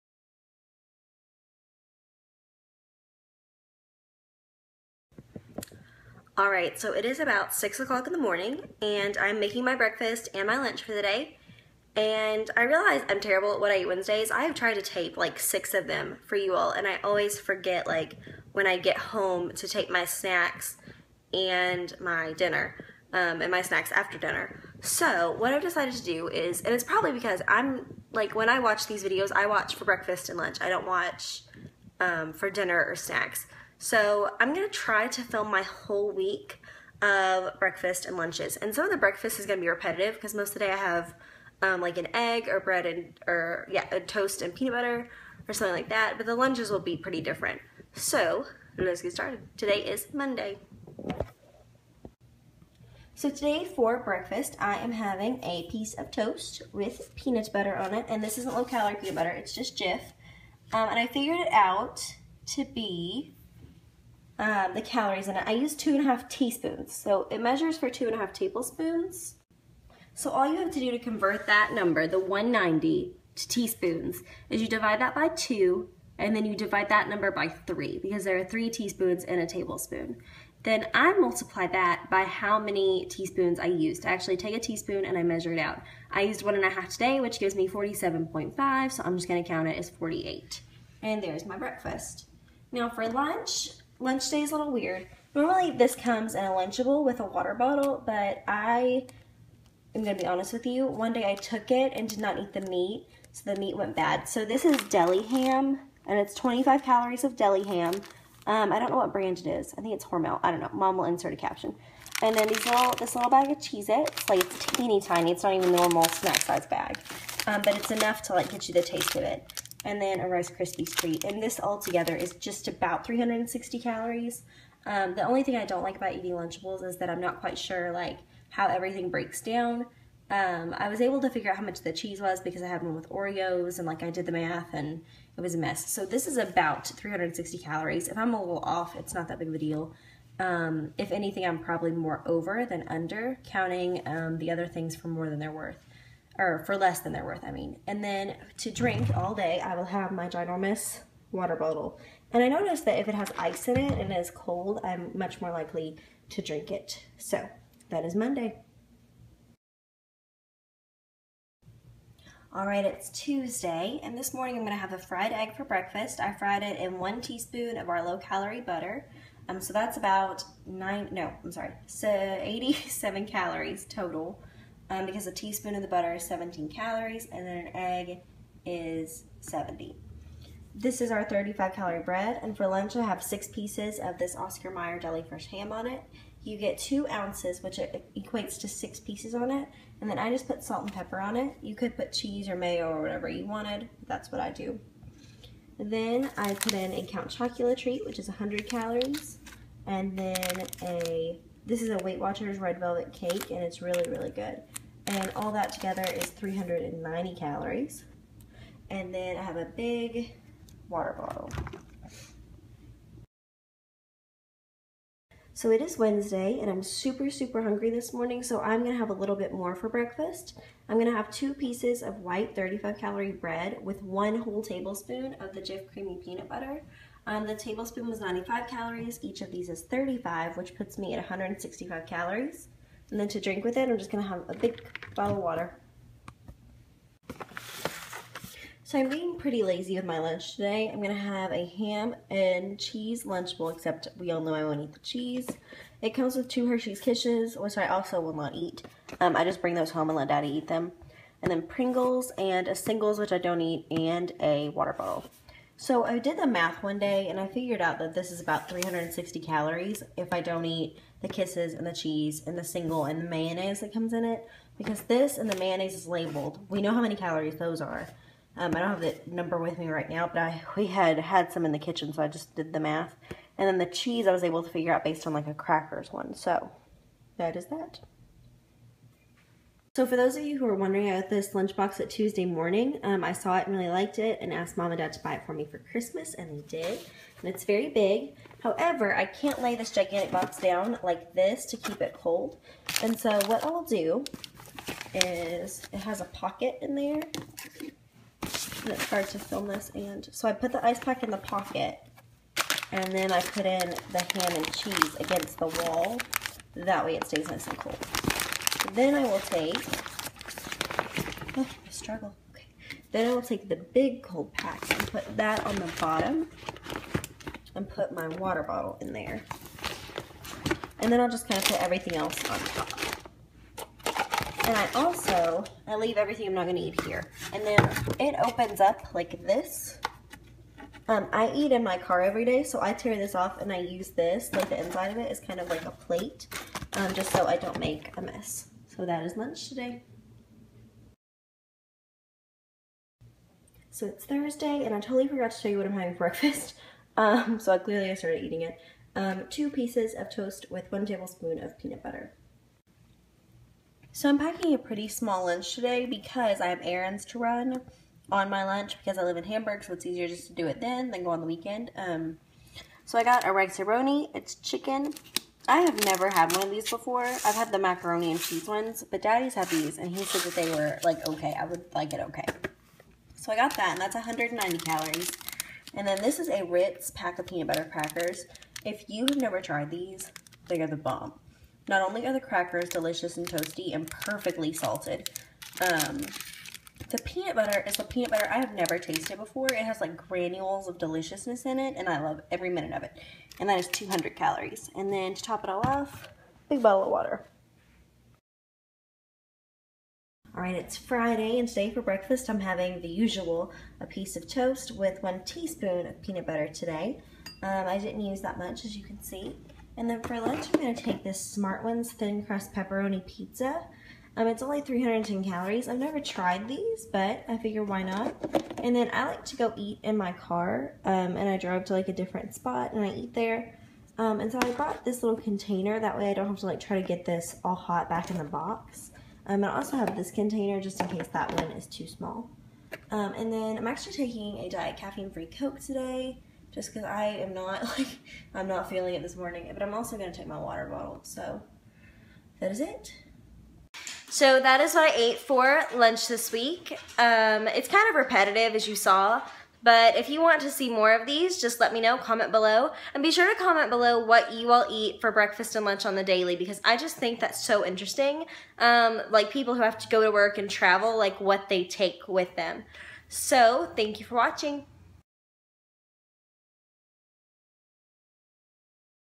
Breakfast & Lunch 12/7/15 – 12/11/15
Since I can't seem to film an entire day for What I Ate Wednesday, I figured I would film my breakfast and lunch for an entire week. I hope you enjoy it!